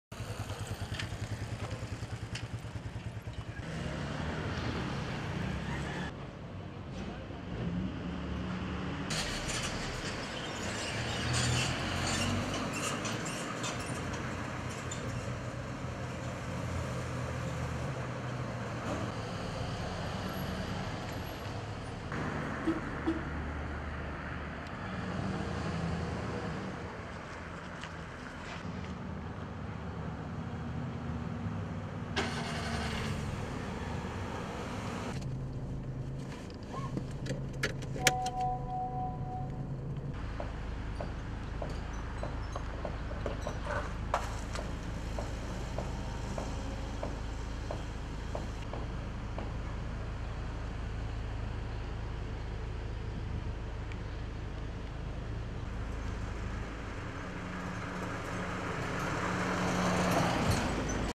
Piden colocación de topes en la calle Argentina
Habitantes de la Calle Argentina en la colonia Francisco I. Madero piden la colocación de topes ya que señalan que desde que pavimentaron esa calle la utilizan como autopista.

Otros vecinos comentan que se requiere de la colocación de un semáforo porque los que transitan por la calle Argentina al llegar a la Rómulo Díaz no hacen alto, o lo hacen muy adentro de las vialidades con riesgo de accidente...